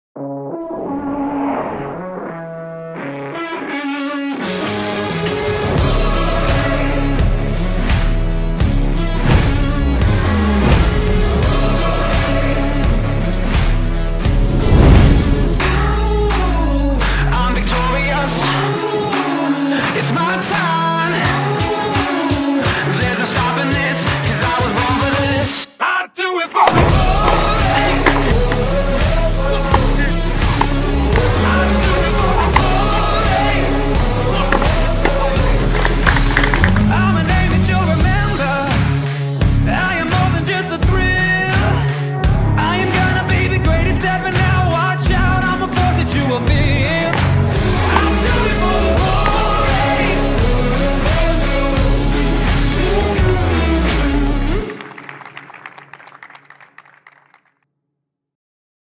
TECHSET OPEN PRO 2020
Techset Open Pro realizado na Techset Academy, pagou R$3.000,00 para o campeão, tendo sua final disputada por Guilherme Clezar e Pedro Sakamoto.

Apoio: Martins e Souza Advogados, Coco Legal e Magikk